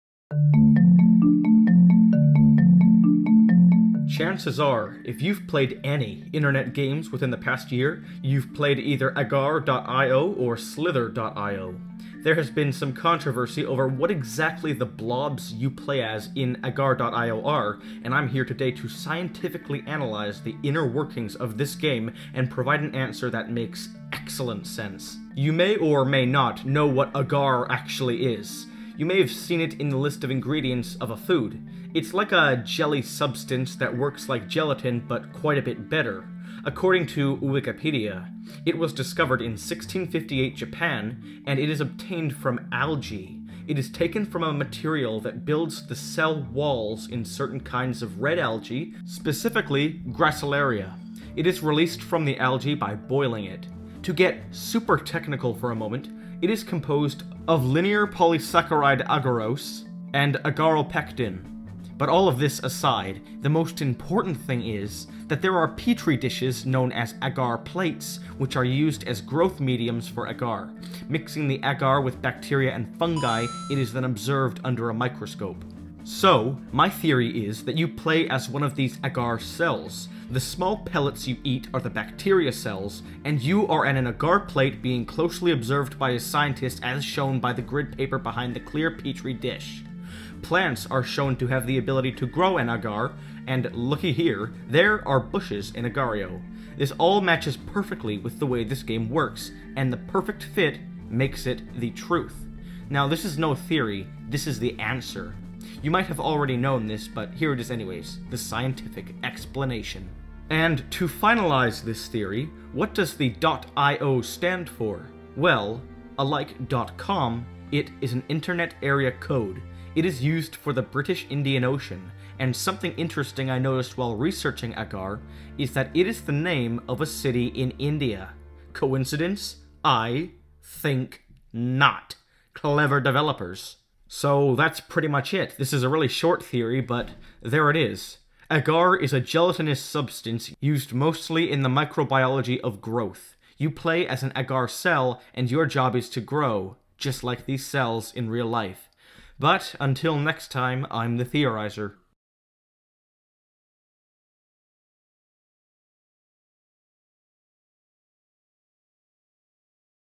The Scientific TRUTH About Agar.io! [Theory]
NOTE: If you were wondering, the city I mentioned IS Agar. You may know The other and much larger Indian City, Agra. They are two different things. However, I have not been to either, and I believe that when I researched a picture of Agar, India, it gave me the one of Agra.

Agar.io or Agario is probably the most searched up online game of the past year, along with Slither.io or Slitherio. This theory explains the substance you play as in Agario, the stage you play on, and some scientific properties of algae you might not have known!

WHATEVER TOPIC THE THEORY IS ON WAS NOT CREATED BY ME. ONLY THE THEORY WAS, HOWEVER I HAVE RIGHTS TO THIS VIDEO. I DID NOT CREATE THE MUSIC EITHER. PICTURES ARE MOSTLY FROM GOOGLE.